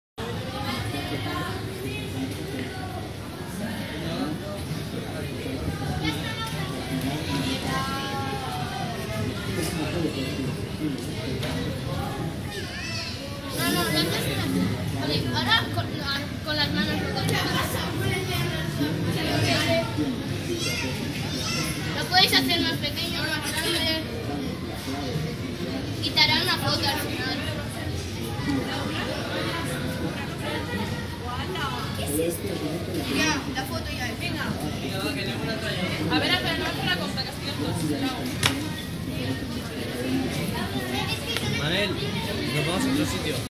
Realidad virtual
Fijaos si nuestros peques están ya puestos al día con la tecnologia, que cuando se cuelga la aplicación salta un niño del público y la restaura